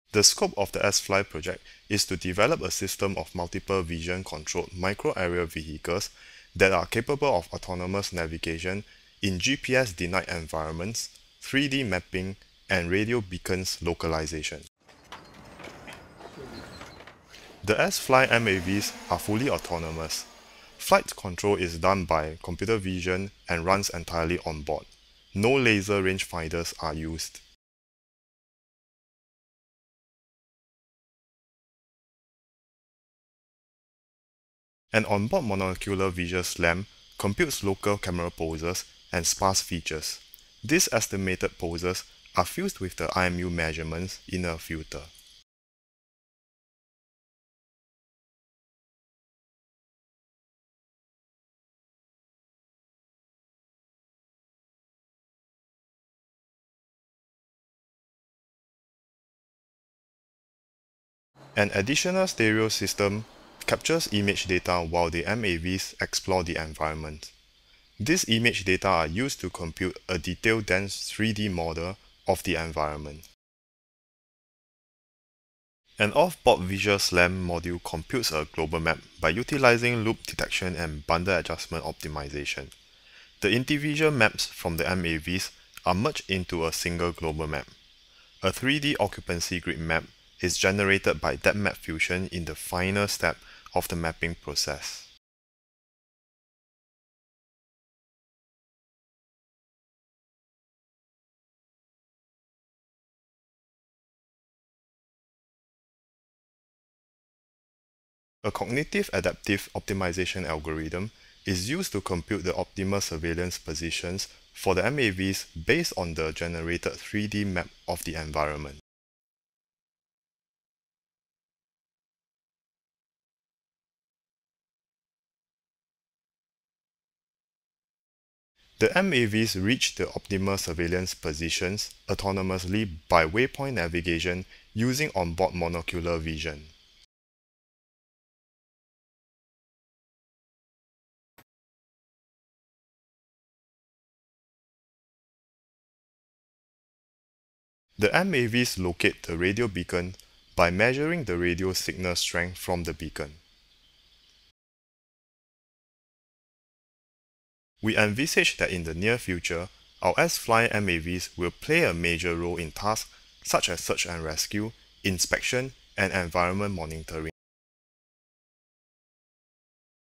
SFly: Swarm of Micro Flying Robots (Finalist best video paper award IROS 2012)
The scope of the sFly project (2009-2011) was to develop a system of multiple vision-controlled micro aerial vehicles that are capable of autonomous navigation in GPS-denied environments, 3D mapping, and radio-beacons localization. The Sfly MAV's are fully autonomous. Flight control is done by computer vision and runs entirely onboard. No laser rangefinder or GPS are used, but just onboard cameras running at 30 Hz. An on-board monocular visual slam computes local camera poses and sparse features. These estimated poses are fused with the IMU measurements in a filter. "An additional stereo system captures image data while the MAVs explore the environment. These image data are used to compute a detailed dense 3D model of the environment. An off-board visual SLAM module computes a global map by utilizing loop detection and bundle adjustment optimization. The individual maps from the MAVs are merged into a single global map. A 3D occupancy grid map is generated by depthmap fusion in the final step of the mapping process. A cognitive adaptive optimization algorithm is used to compute the optimal surveillance positions for the MAVs based on the generated 3D map of the environment. The MAVs reach the optimal surveillance positions autonomously by waypoint navigation. The MAVs locate the radio beacon by measuring the power of the received radio signal (RSSI value). We envisage that in the near future our sFly MAVs will play a major role in tasks such as search and rescue, inspection, and environment monitoring.